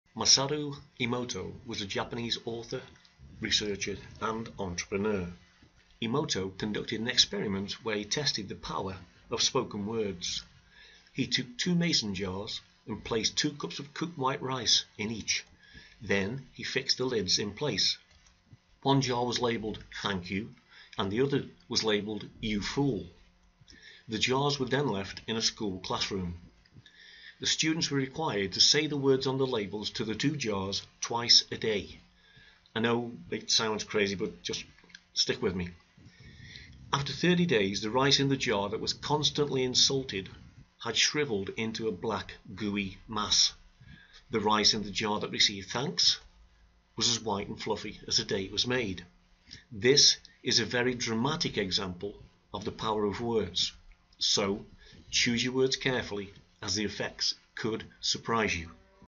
Your Words Have Power
Your words have power is about the effect you can create with the spoken word